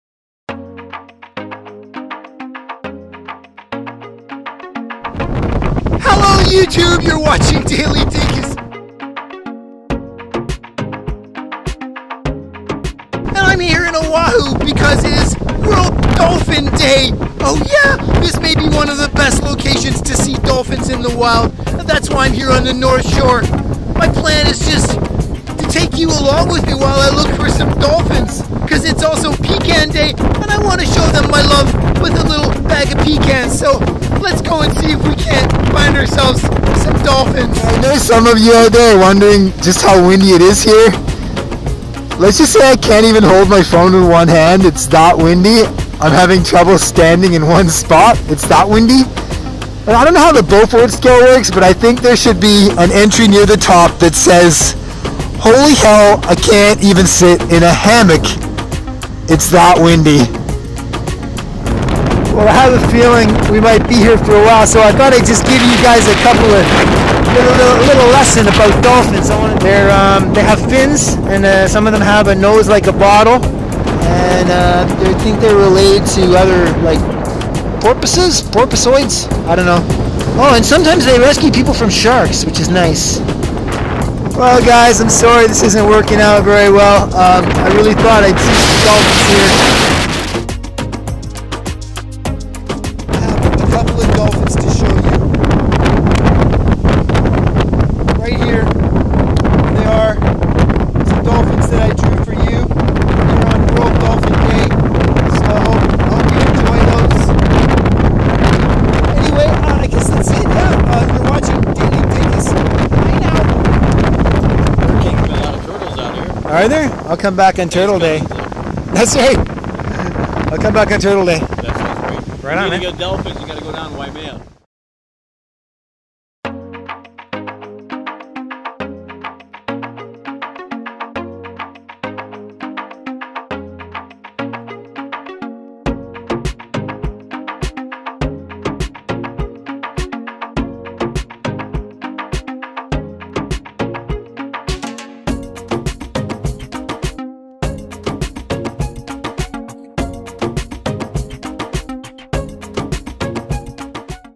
Daily Dinkus in Hawaii - April 14 is Dolphin Day
I travel the world celebrating silly holidays, every day.
For today's video:
April 14th is Dolphin Day so I'm in Oahu looking for dolphins...